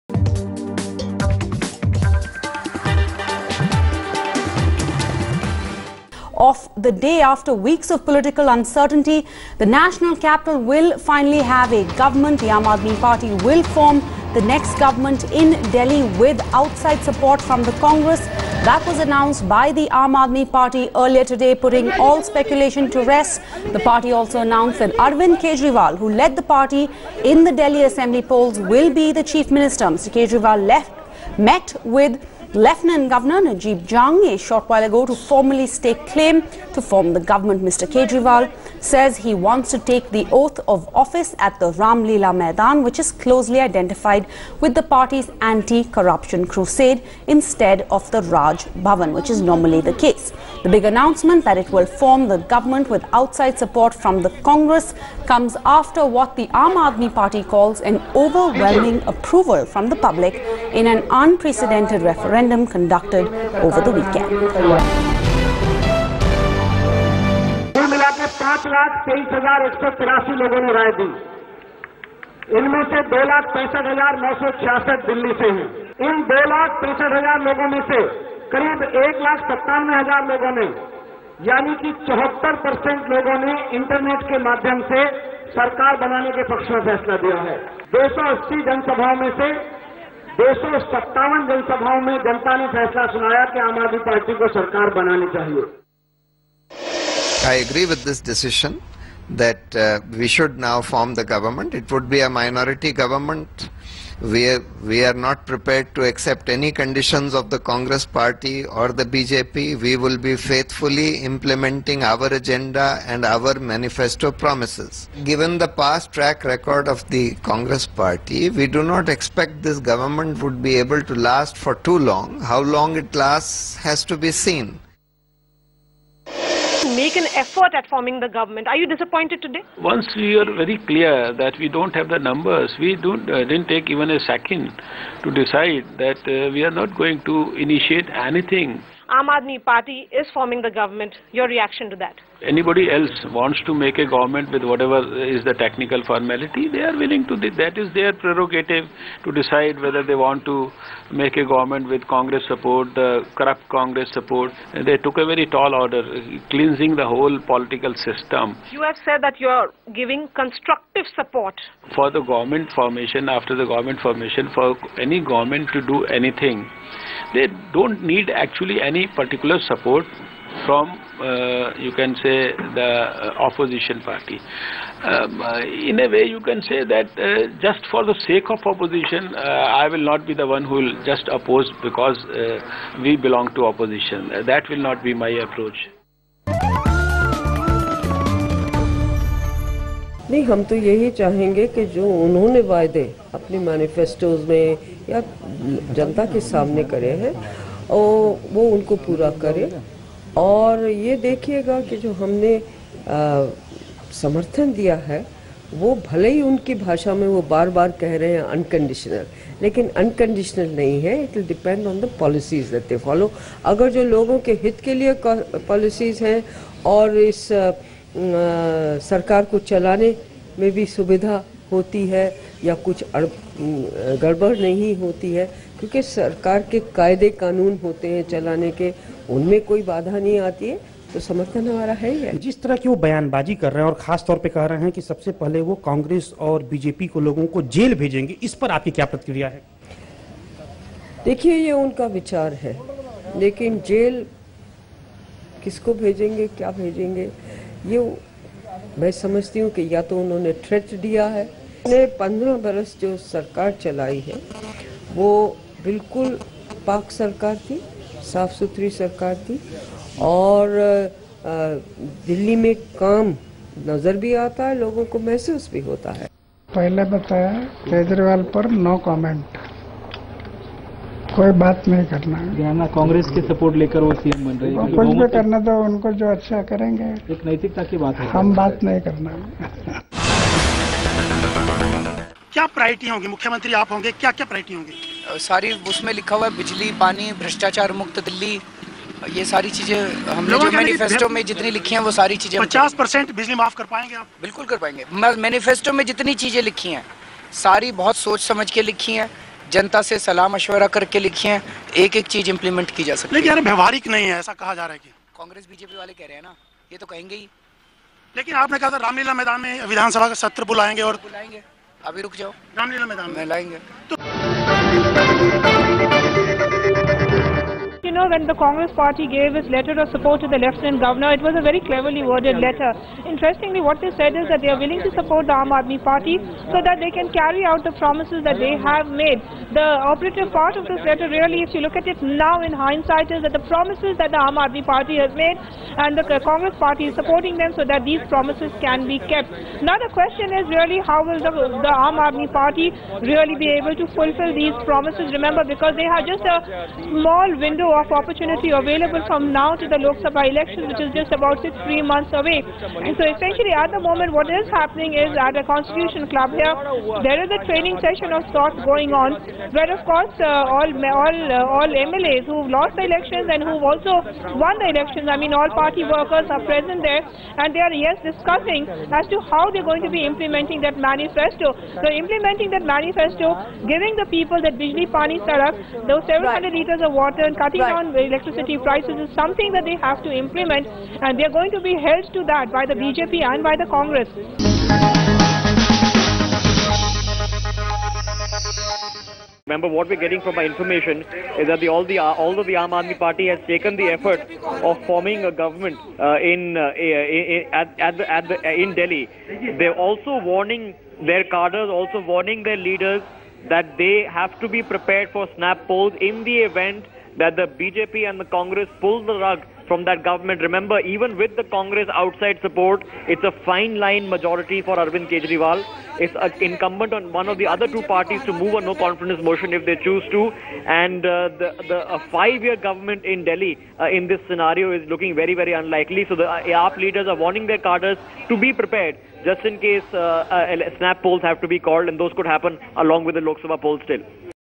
AAP set to form government in Delhi
The Aam Admi Party (AAP) announced that it would form the government in New Delhi, with founder-leader Arvind Kejriwal as the Chief Minister of Delhi. Party leader Manish Sisodia said that 74% people in Delhi favoured AAP forming the government.